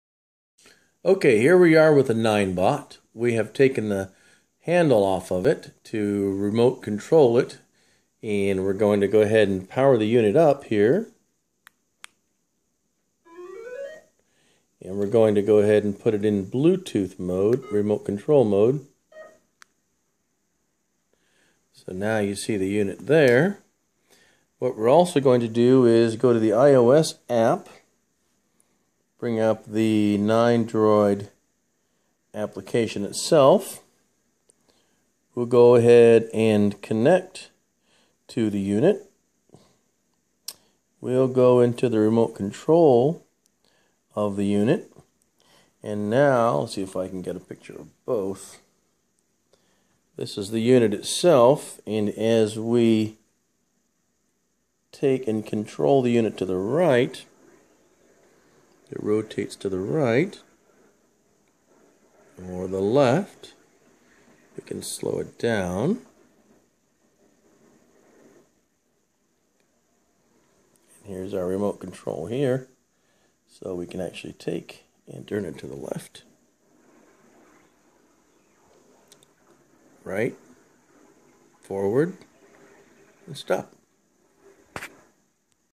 Ninebot being remotely controlled with IOS app
Here is the Ninebot Personal Transporter being controlled remotely using an iPad